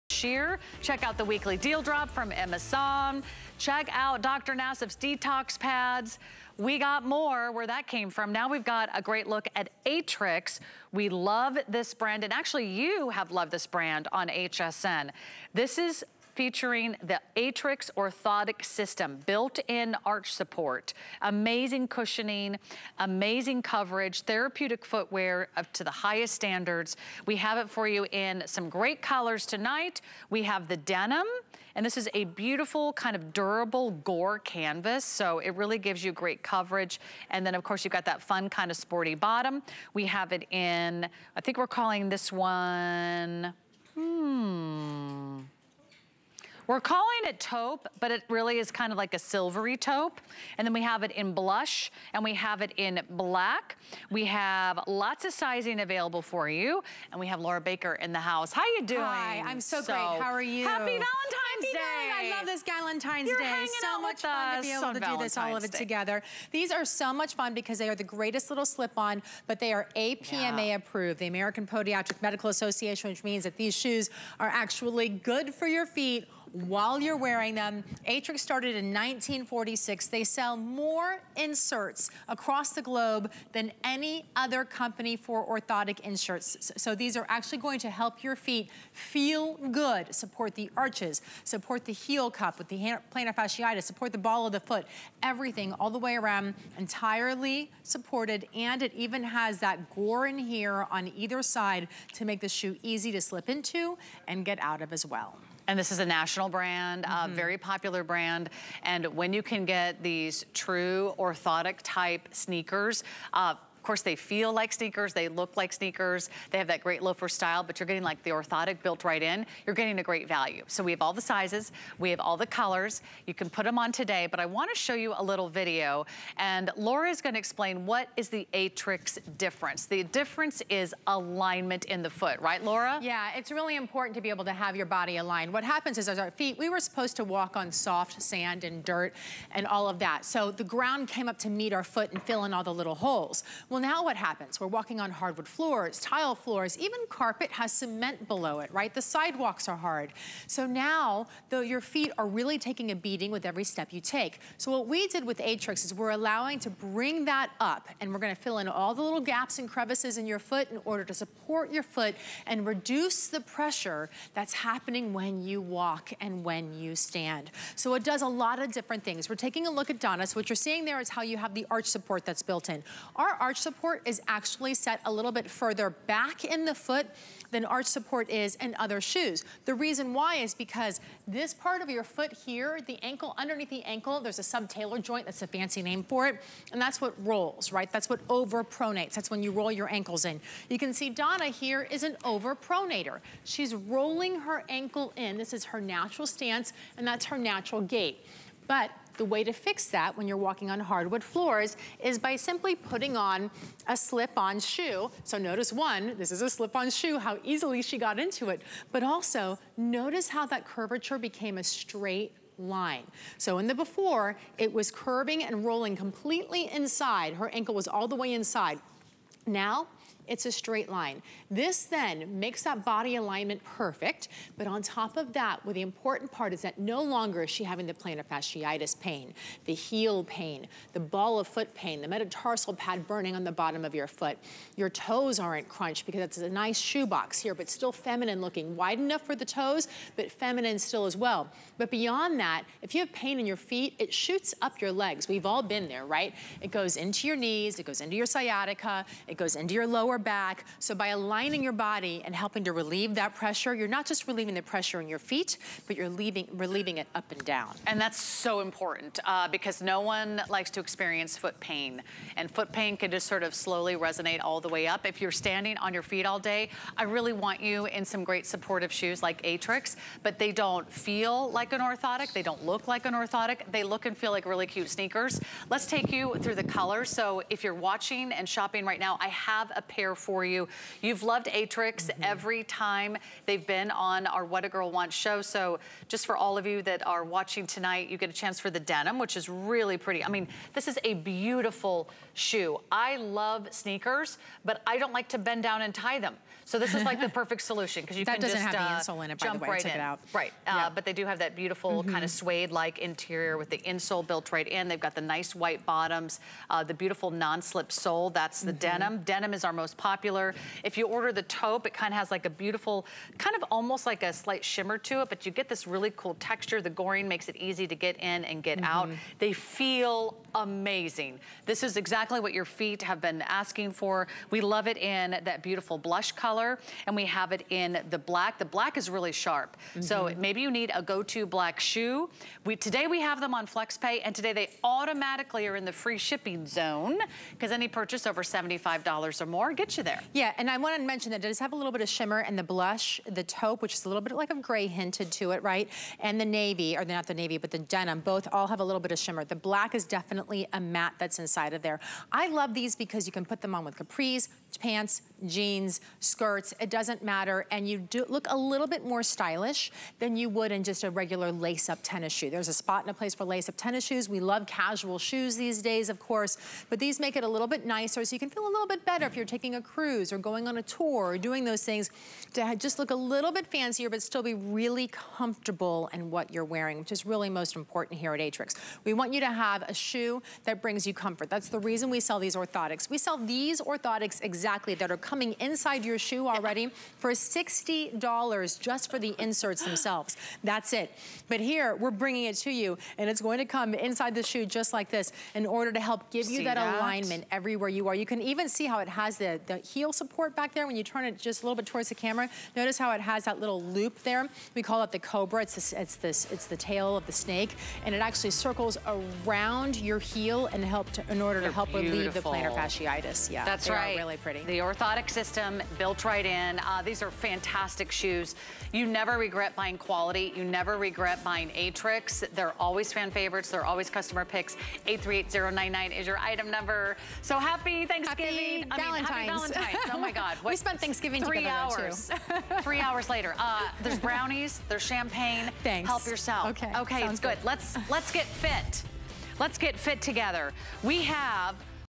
Aetrex Cameron SlipOn Canvas Sneaker
This fashionable easytowear sneaker, with arch support, provides the lift you need on long days.